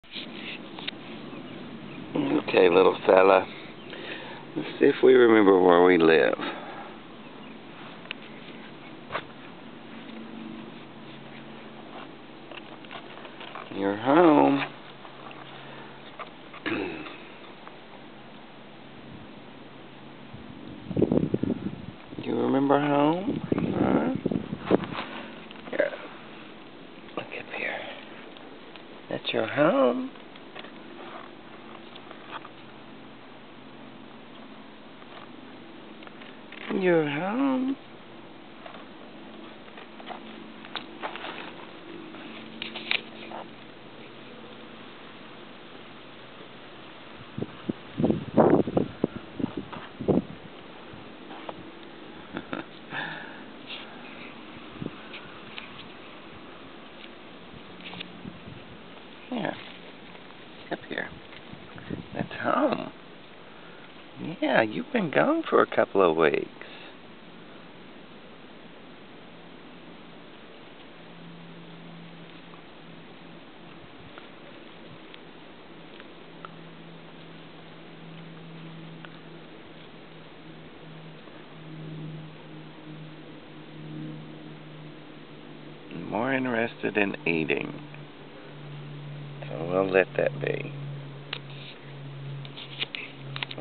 Two of the Baby Gophers Tortoises come home from the vet with a clean bill of health
After being trapped under water in their burrows and spending nearly 2 weeks at the vet silvers babies come home with a clean bill of health and are URTD free.